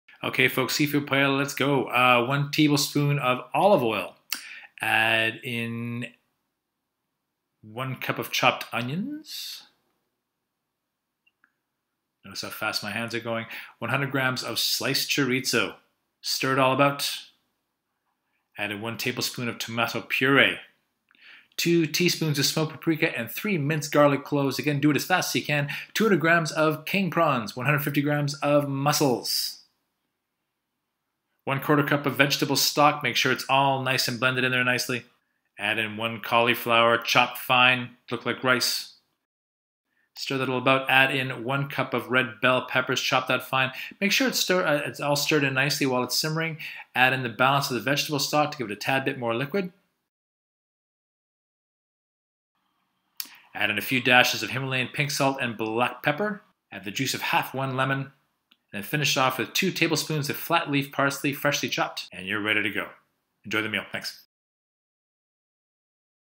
Fit to Last Seafood Paella - gluten-free goodness!
Clapham personal trainer James Staring from Fit to Last shows you how to make a yummy seafood paella with cauliflower rice…yum!